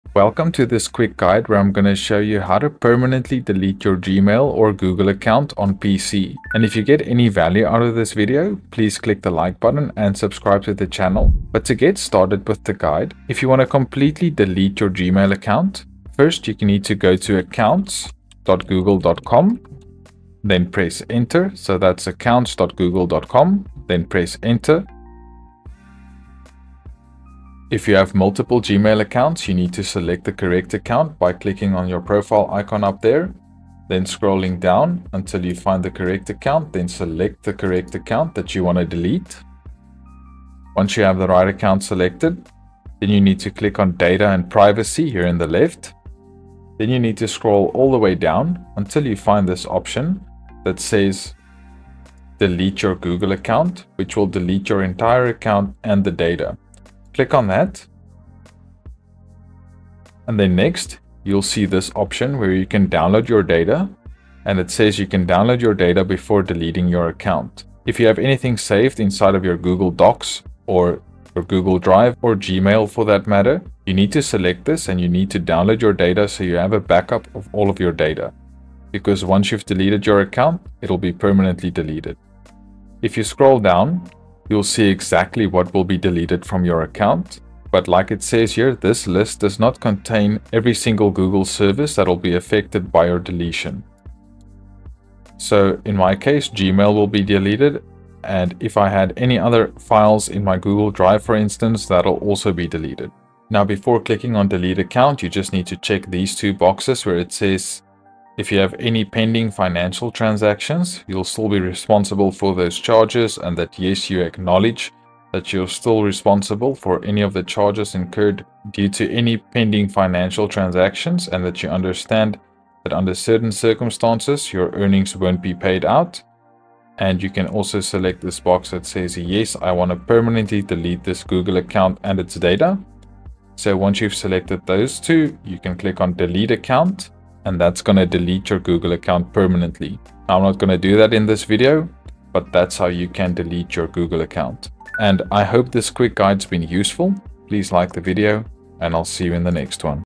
How to Delete a Gmail Account Permanently on PC (2024) [Tutorial]
In this tutorial video, I'll show you how to delete a gmail account permanently on pc. So, if you've been wondering how you can delete your gmail account, this video is for you as I'll take you through the entire process step-by-step.